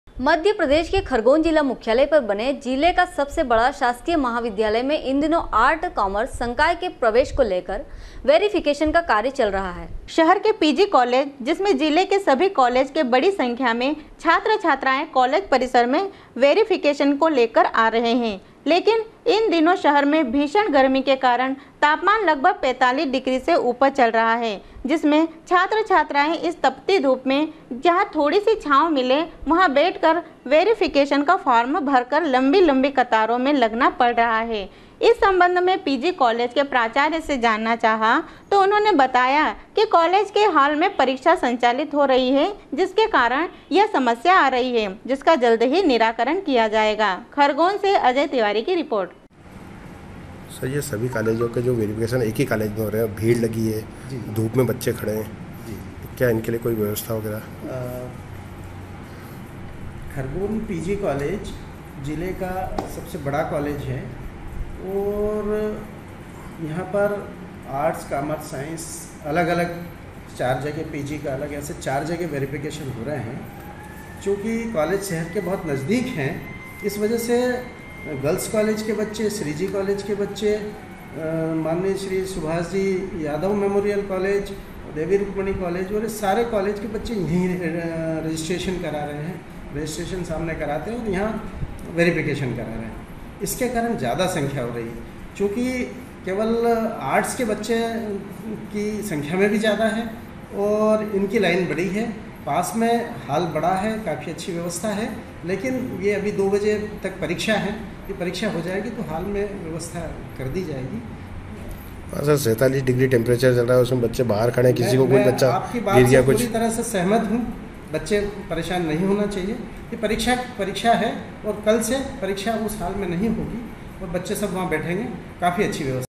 sw24news khargone, 13.06.2019
मध्यप्रदेष के खरगोन जिला मुख्यालय पर बने जिले का सबसे बड़ा शासकीय महाविद्यालय में इन दिनो आर्ट, काॅमर्स संकाय के प्रवेष को लेकर वेरिफिकेषन का कार्य चल रहा है